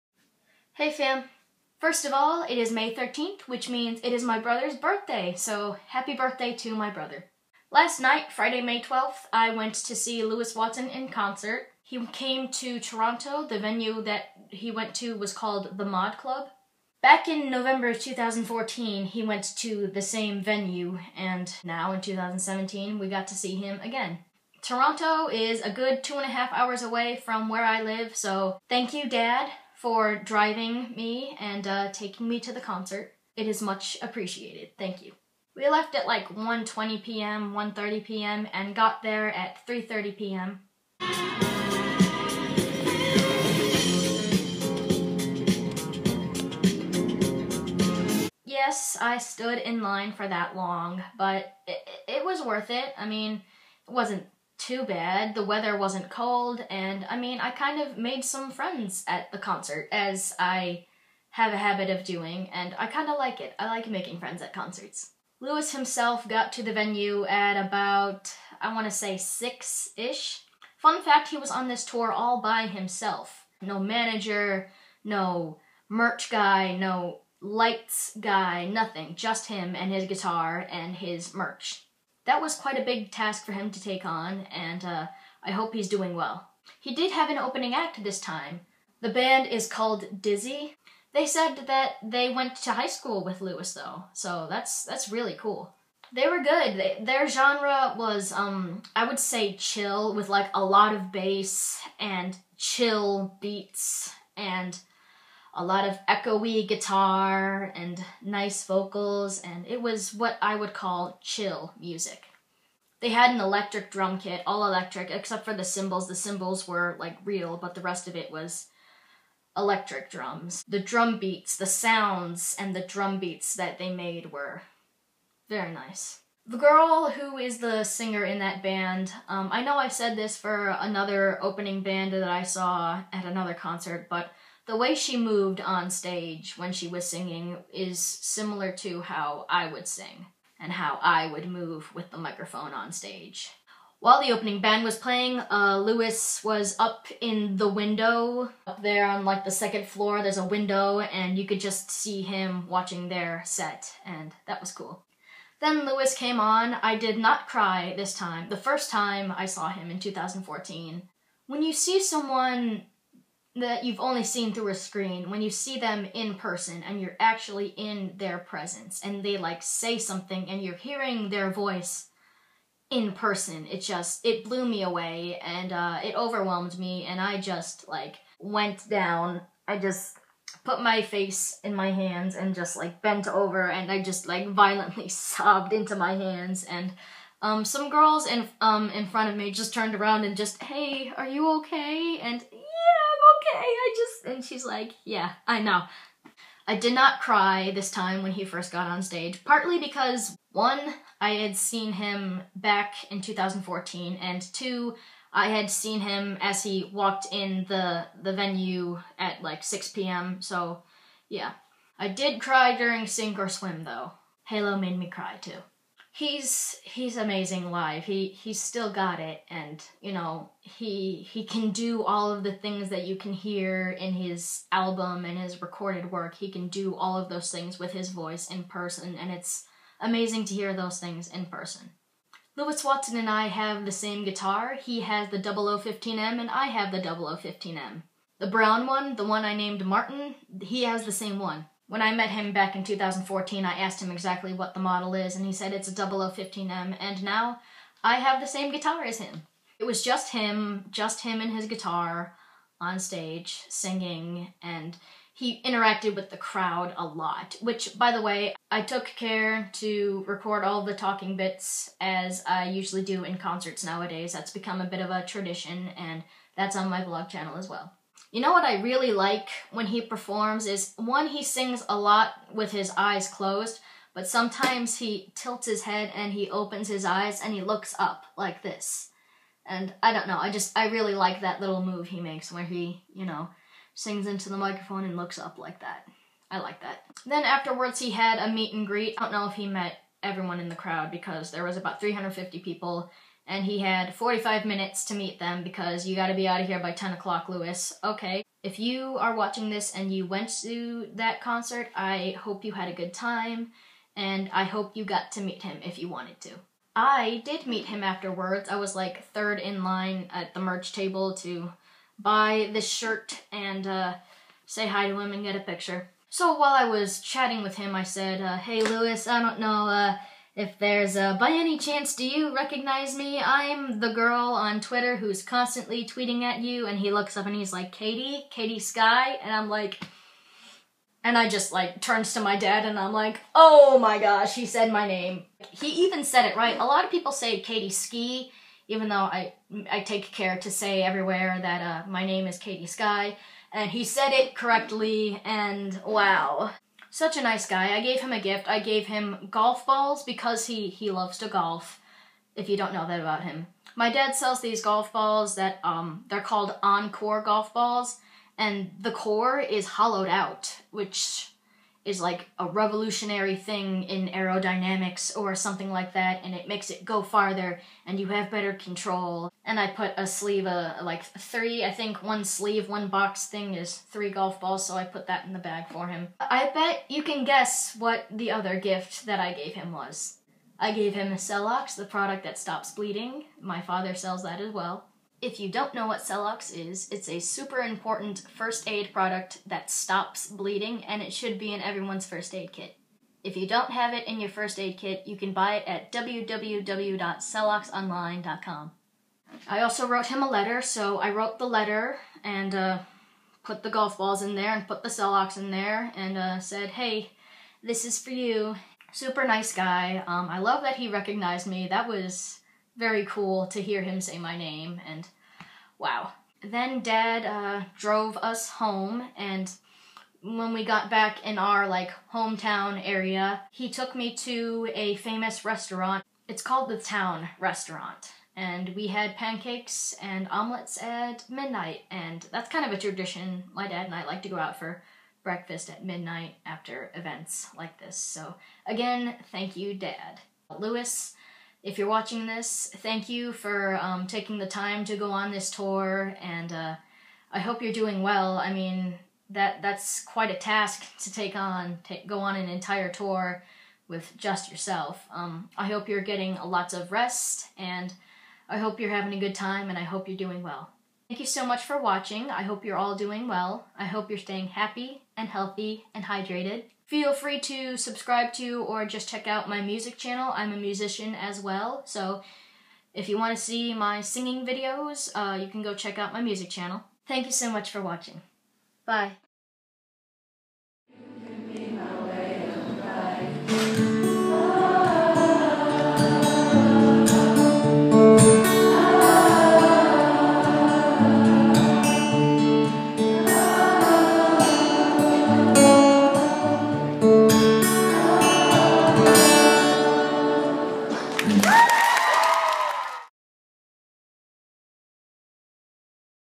Concert vlog: Lewis Watson
I went to see Lewis Watson on May 12, 2017 at the Mod Club in Toronto. I recorded everything he said at the concert and you can watch that

Celox first aid products (this should be in everyone's first aid kit)